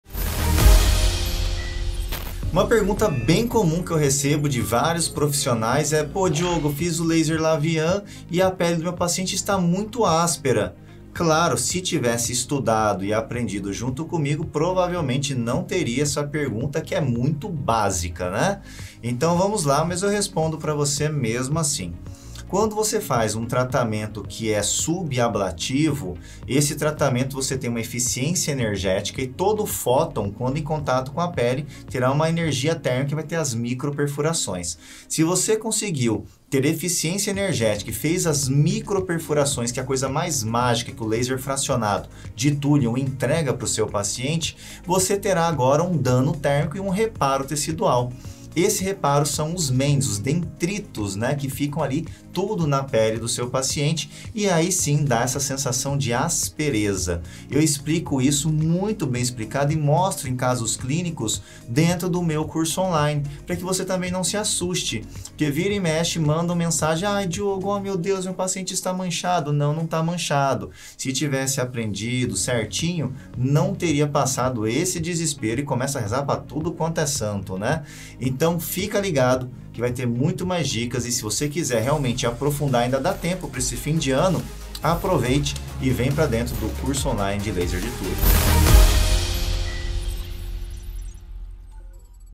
LASER LAVIEEN - DICA #05
Você sabe porque a pele do paciente pós Laser Lavieen fica “áspera”?

Sabe exatamente como o Laser de Thulium age na pele do seu paciente?

E quais cuidados deve prescrever para o home care desse paciente?

Eu te explico tudo isso no meu curso online de Laser de Thulium, clique no link e confira!